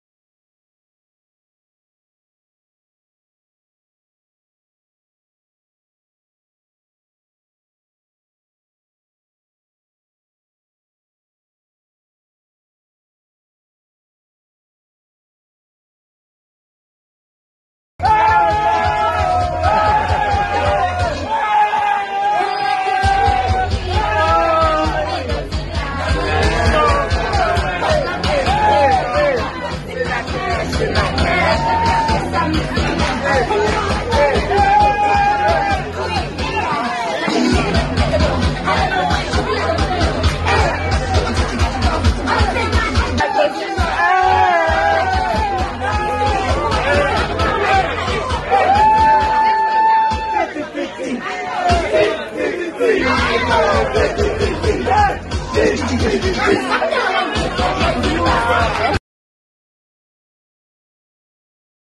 Sho Madjozi forced to danse in Ivory Coast 💃🏾
South African music phenomenon Sho Madjozi had to improvise a dance in Ivory Coast 💃🏾